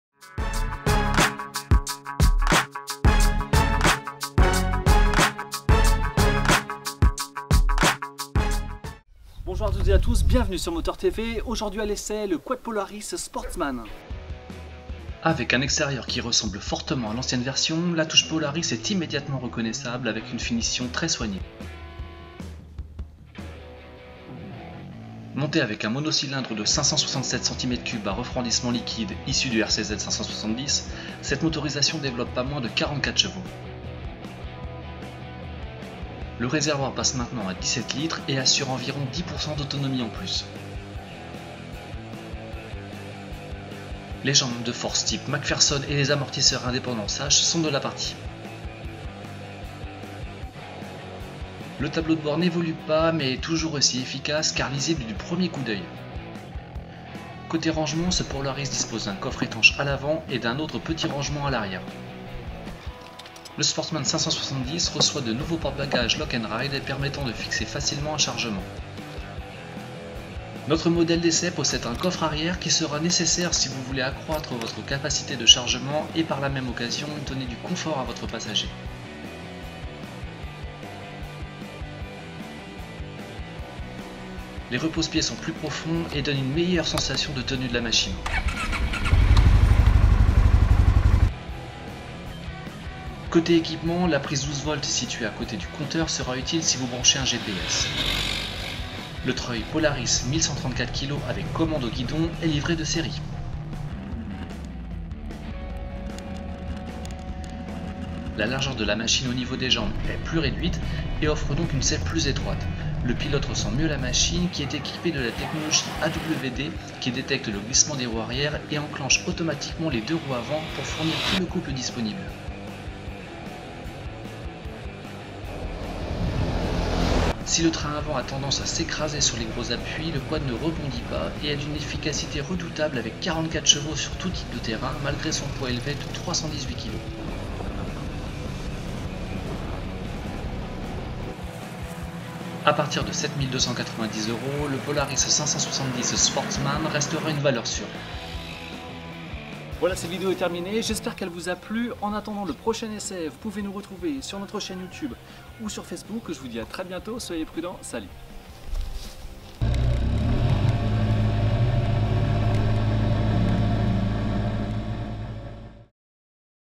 Quad Polaris 570 Sportsman / 2016 / MoteursTv
Essai du Quad Polaris Sportsman de 44 ch
Test drive / review
Essai en français du Quad Polaris Sportsman version Forest 2016 qui avec son poids éleve de 318 kilos, passe la majorité des obstacles sans broncher avec ses 44 cv et grâce à la technologie AWD qui détecte automatiquement le glissement des roues arrières et enclenche les deux roues avant pour fournir tout le couple disponible.
La description de tout ce que vous devez savoir sur ce véhicule, et son essai sur route pour avoir un point de vue objectif sont dans cette vidéo. A bientôt !
Lieu de tournage : Vimoutiers

Nicolas